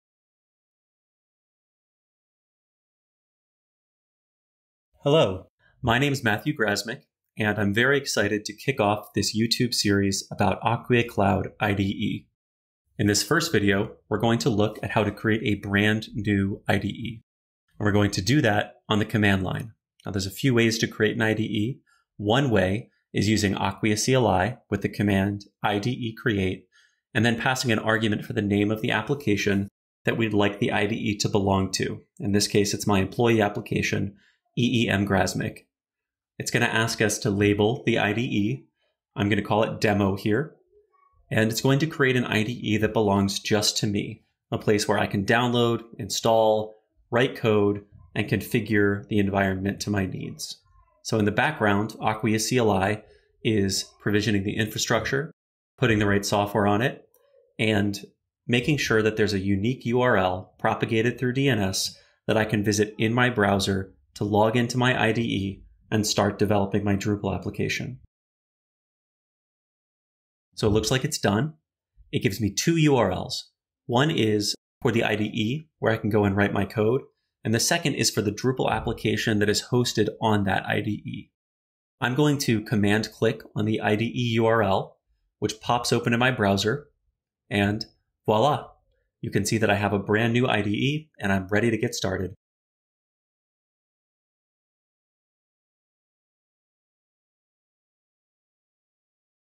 Creating a New IDE - Cloud IDE Miniseries
In this video, we will demonstrate how to create a new IDE with Acquia Cloud IDE.